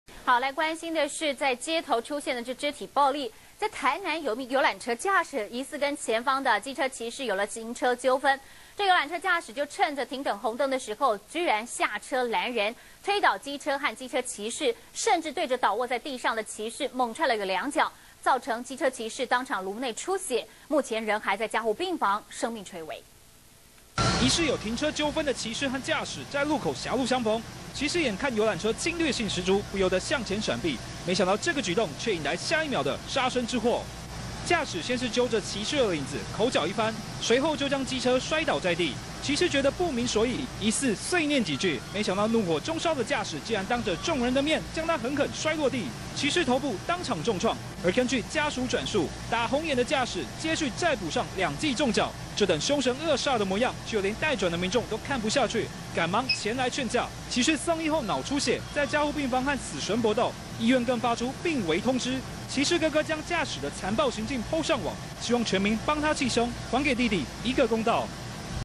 中視新聞》行車糾紛！ 遊覽車駕駛推翻機車毆騎士
中視新聞》又是當街上演的肢體暴力！台南一名遊覽車駕駛，疑似和前方的機車騎士有行車糾紛，竟然趁著紅綠燈下車攔人，不但推倒機車和騎士，還疑似對著倒臥在地的騎士，猛踹兩腳，騎士當場顱內出血，目前還在加護病房，生命垂危。